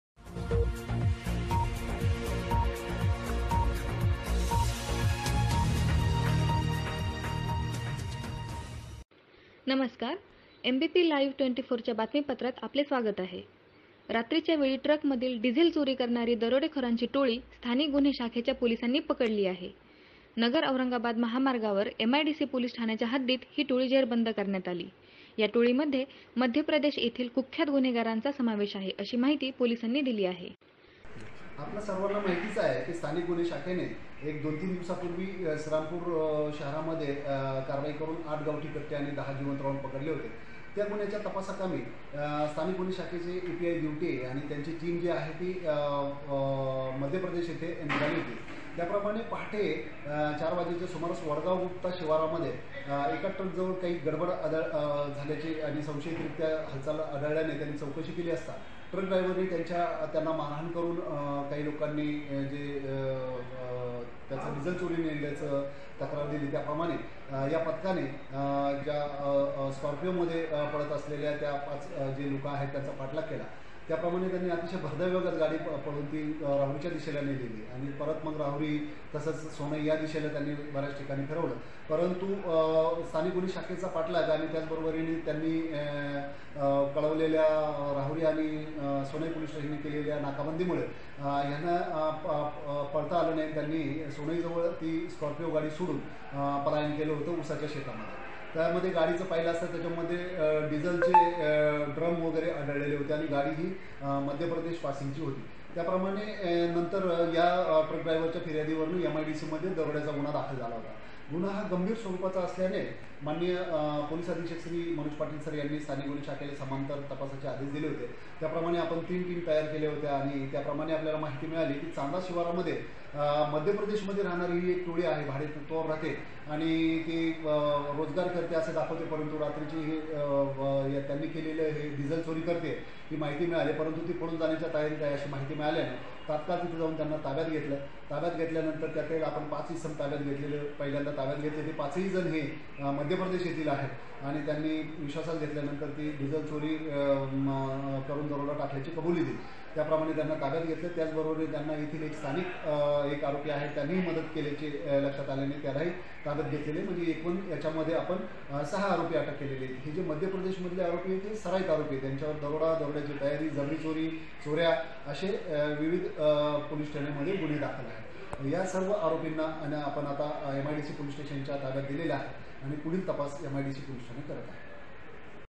ट्रकमधून डिझेल चोरी करणारी टोळी जेरबंद | Crime Branch Ahmednagar
अहमदनगर - रात्रीच्या वेळी मालवाहतुक करणाऱ्या ट्रकमधील डिझेल चोरी करणारी दरोडेखोरांची टोळी स्थानिक गुन्हे शाखेच्या पोलिसांनी पकडली आहे.

नगर औरंगाबाद महामार्गावर एमआयडीसी पोलिस ठाण्याच्या हद्दीत ही टोळी जेरबंद करण्यात आली.

या टोळीमध्ये मध्यप्रदेश येथील कुख्यात गुन्हेगारांचा समावेश आहे, अशी माहिती पोलिसांनी दिली आहे.